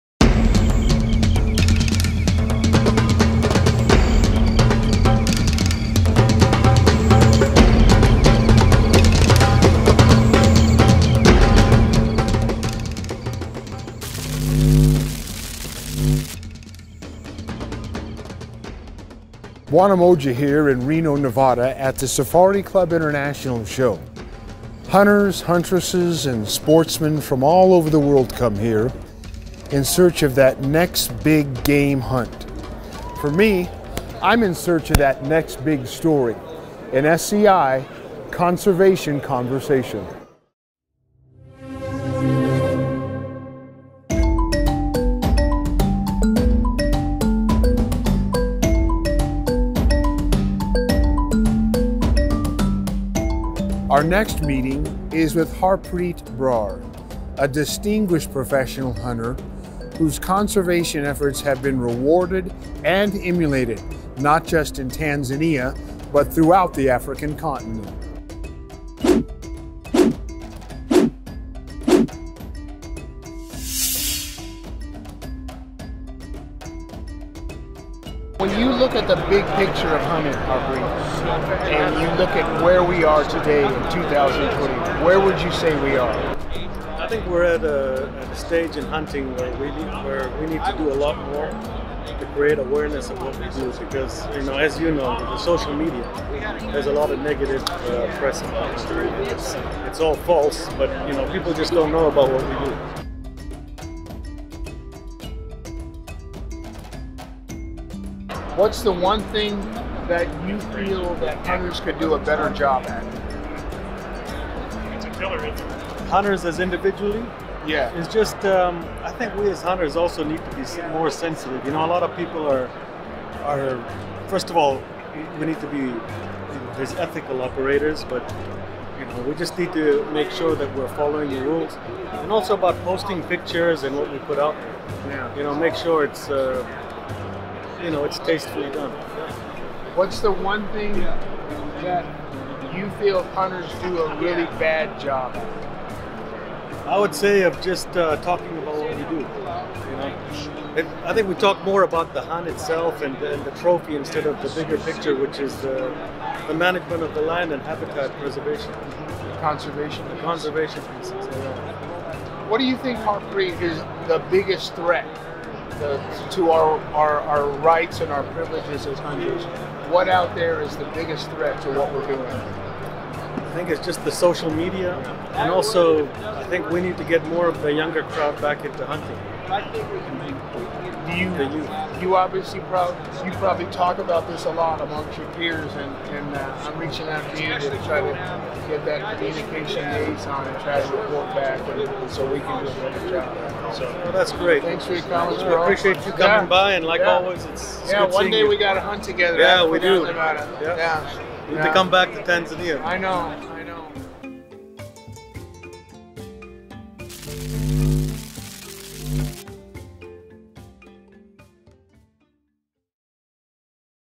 Conservation Conversation: Season 2/Ep 3-PH Harpreet Brar | "We All Can Do Alot More."
SCI's Bwana Moja conducts the latest in his series of conservation conversations with Tanzania PH Harpreet Brar. In this discussion, Harpreet reveals what the outdoors community does right and wrong in the current state of hunting. Tanzania wildlife authorities often seek Harpeet's advice in the ever-changing world of legislation and regulations surrounding the safari industry, given the pulse of hunting which he keeps a close watch on throughout the world. Harpreet supports the promotion of hunting and its benefits via social media platforms but feels sometimes hunters go a bit too far in sharing certain images and videos, which are utilized by the anti-hunting forces on the other side. Harpreet operates from his base, Rungwa Game Safaris on Tanzania's West side